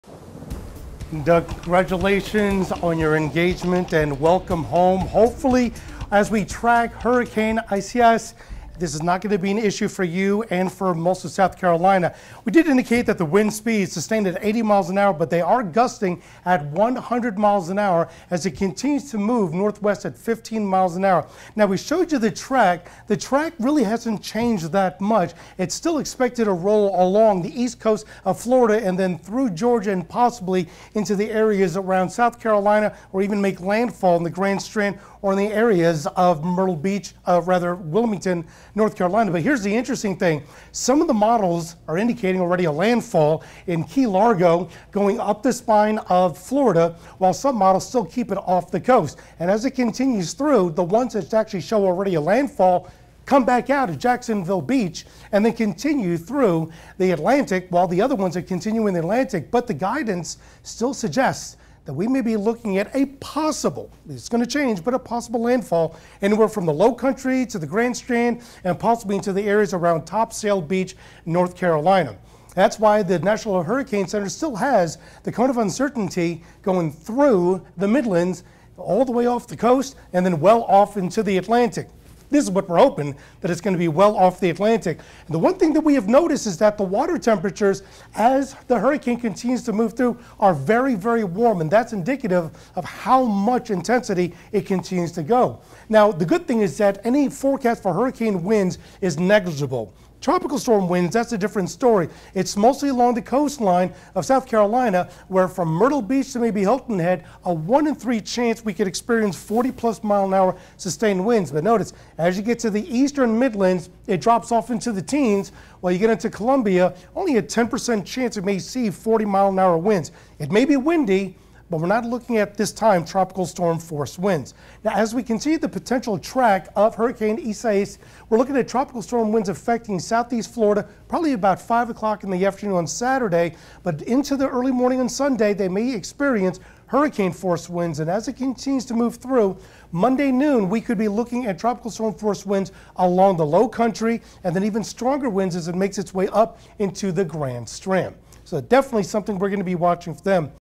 Hurricane Isaias expected to approach Florida coast on Saturday
Hurricane warnings are in effect for Florida as Isaias nears the state's coast as a Category 1 storm. More